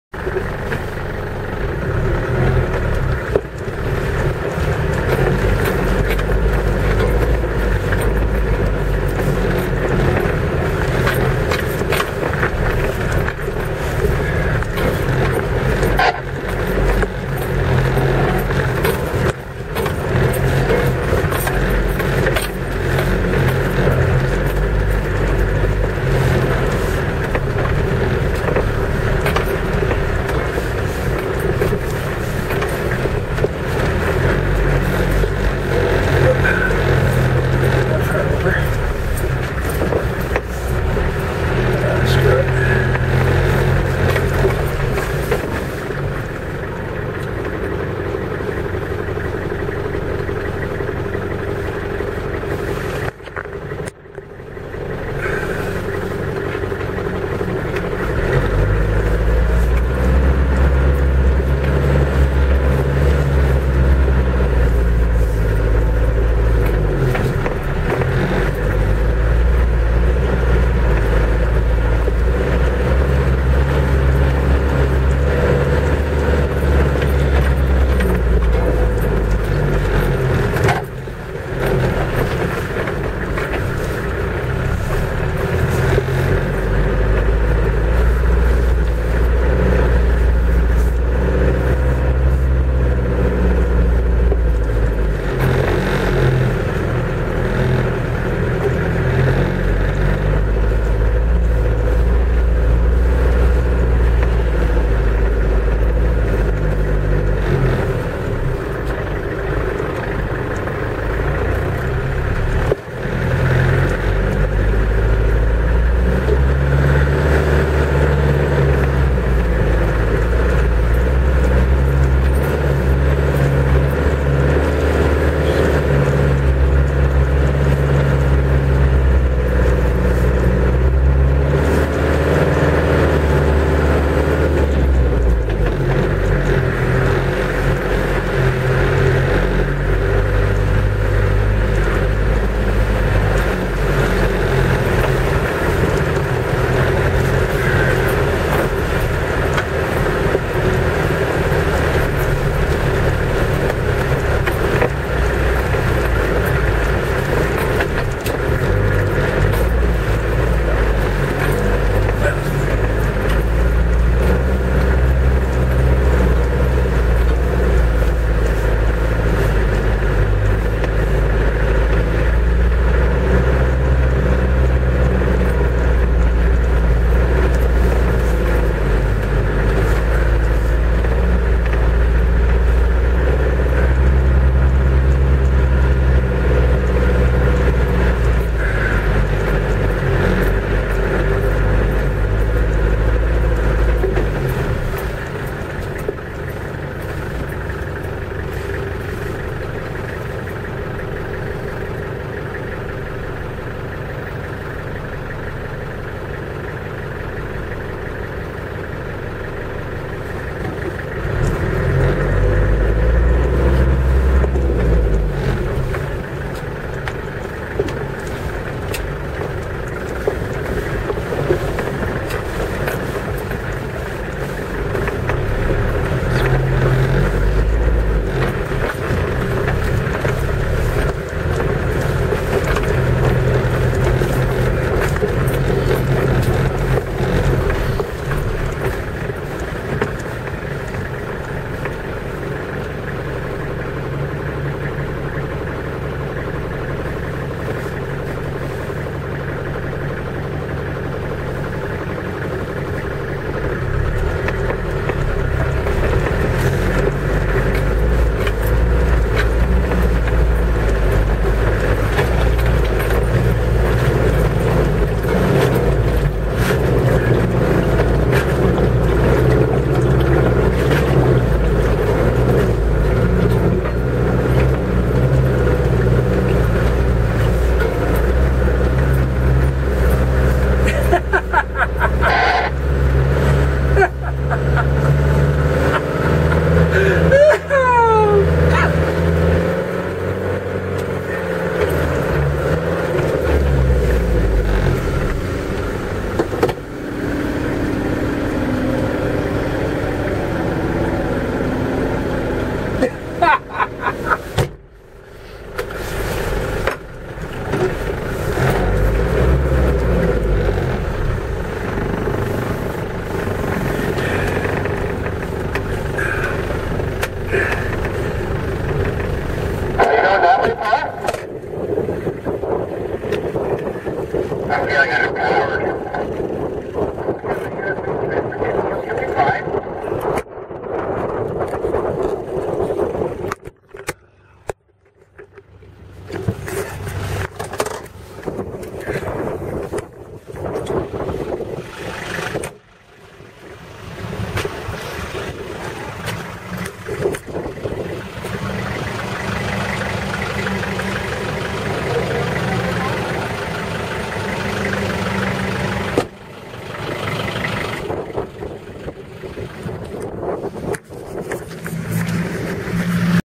Hard and Soft April 21, 2018 DSCN4925
BIG Puddle! A sunny day on the trails! Backcountry 4x4 Club
Sorry about the CB radio clicking! The water was about 4" deep in the cab. That is why I was laughing. I stopped at the end to open the doors to drain out the water. The bow wave was over the headlights and my radiator got clogged with weeds.